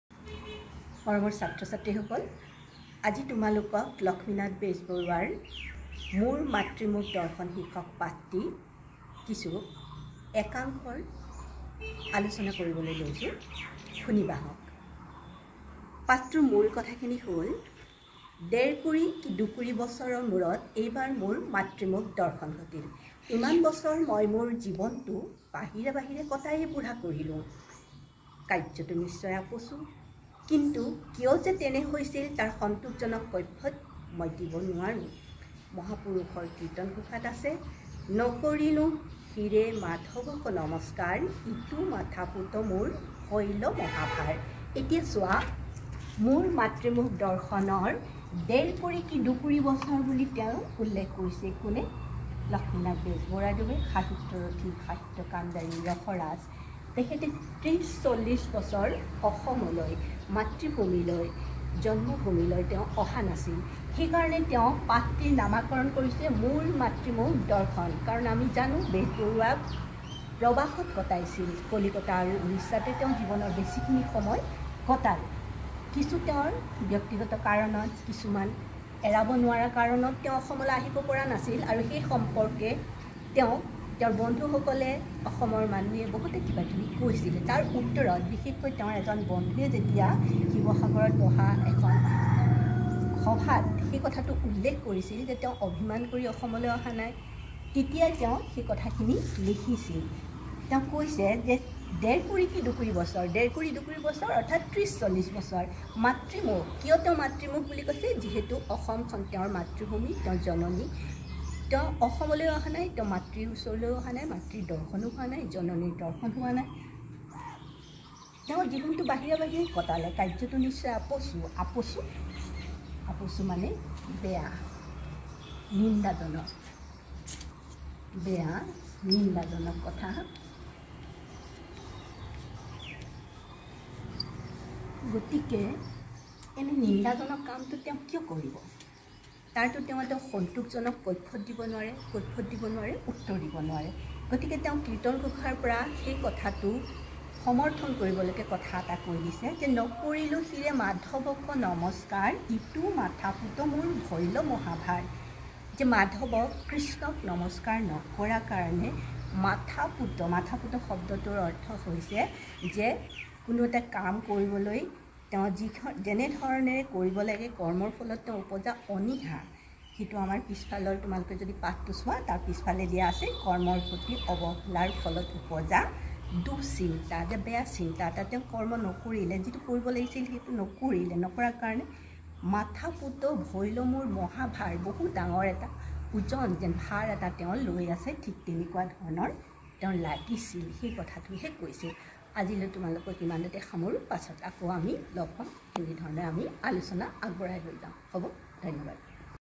Assamese Lesson for class XII by Mrs. N. Gohain.
A lesson by Lakshminath Bezbaruah.
Lecture by Mrs. Nandita Gohain.